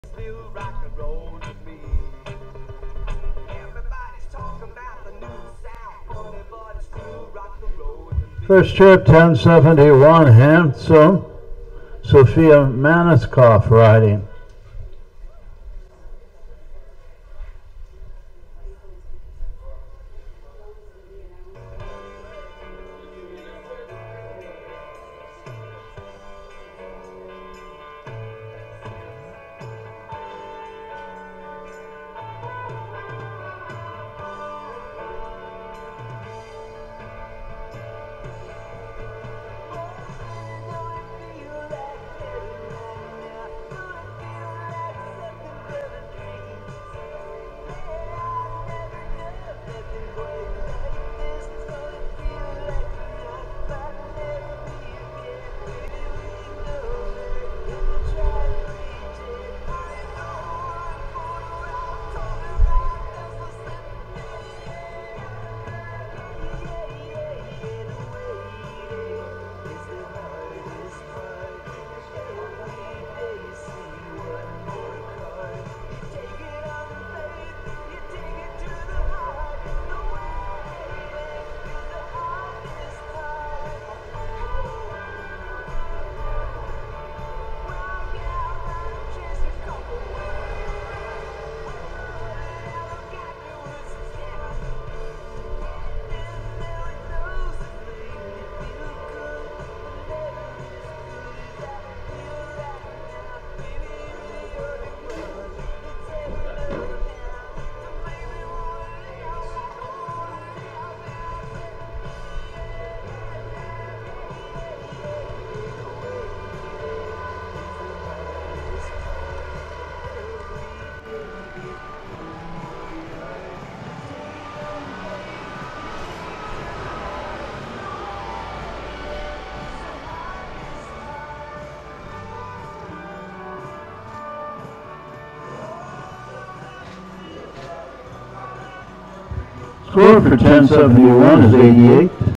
1st Place in MI w Handsome 6/23/24
Sophia Masnikoff
Class of 2024
Handsome DD
1st Place in Low Adult Hunter
Round 1 - Scored 88
Traverse City, MI